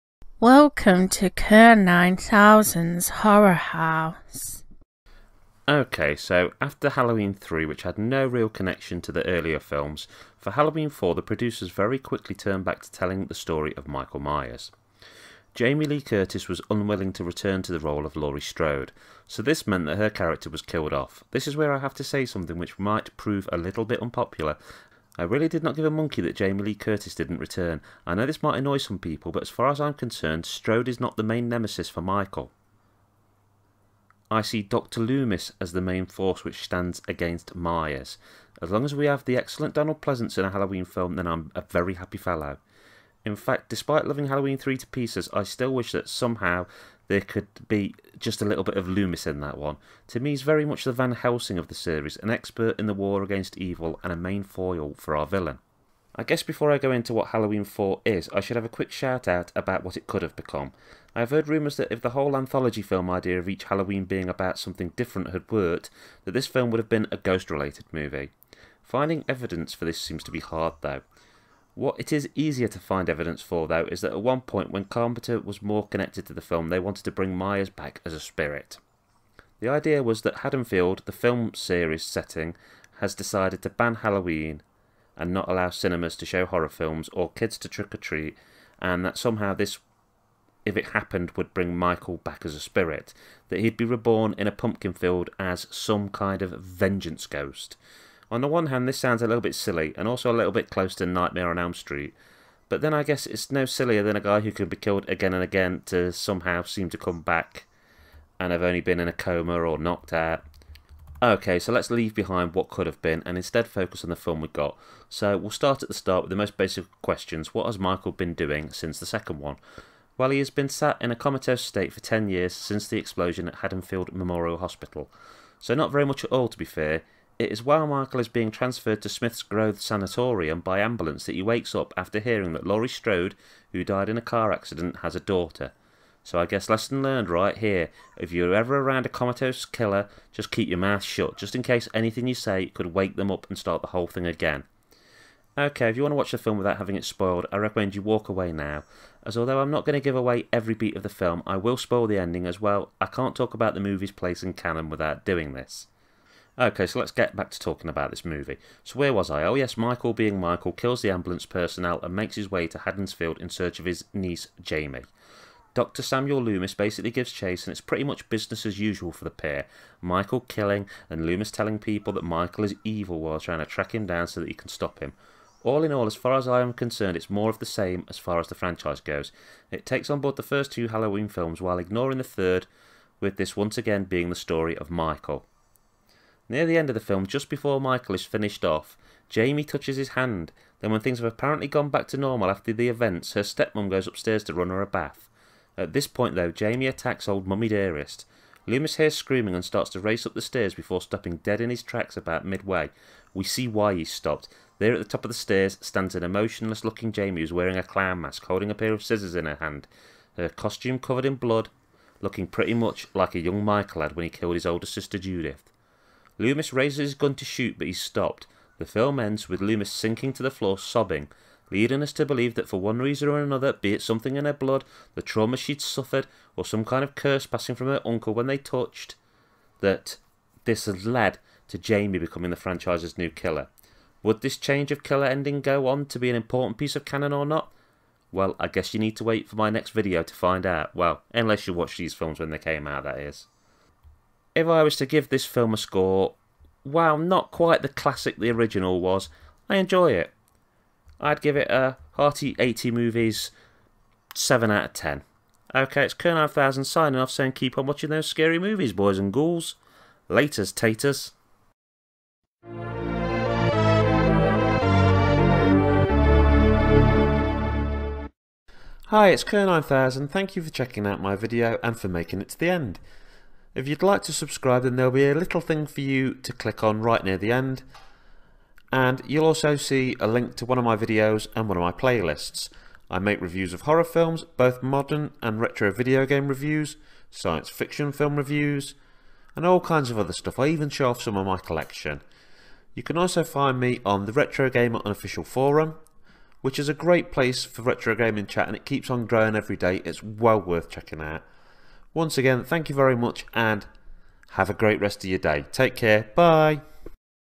Halloween 4 Review and Cannon Discussion
My horror house review of Halloween 4 The Return of Michael Myers